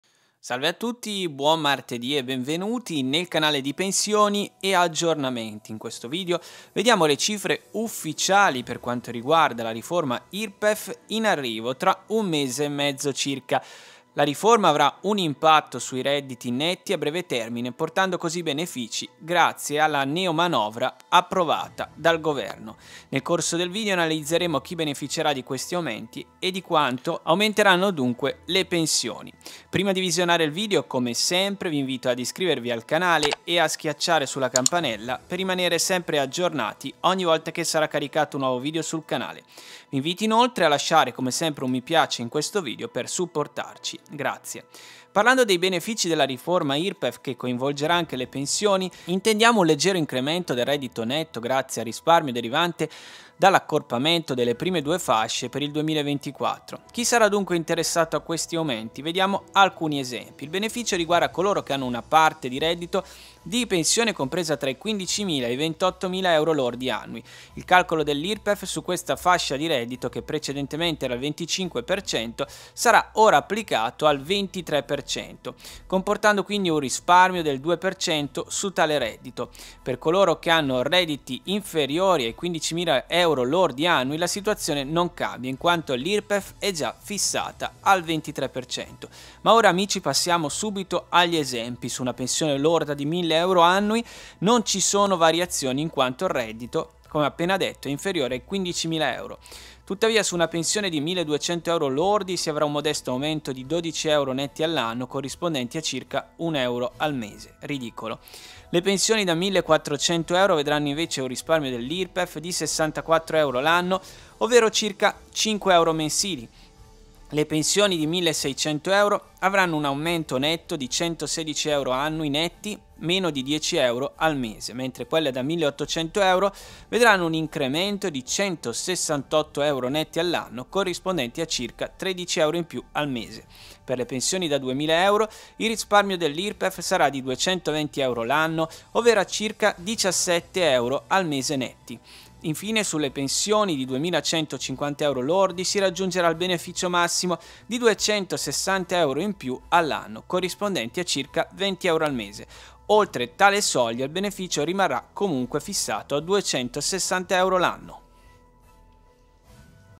PENSIONI AUMENTI NUOVI IMPORTI ESATTI APRILE 2024, RIFORMA IRPEF 📊 TABELLA CON TUTTE LE CIFRE❗️
In questo video esamineremo più da vicino gli incrementi delle pensioni derivanti dalla Riforma dell'IRPEF del 2024, calcolando con precisione gli importi per ciascuna fascia. Esploreremo dettagliatamente tutti gli aumenti previsti e identificheremo anche chi ne beneficerà.

PENSIONI AUMENTI NUOVI IMPORTI ESATTI APRILE, RIFORMA IRPEF 2024 📊 TABELLA CON TUTTE LE CIFRE❗️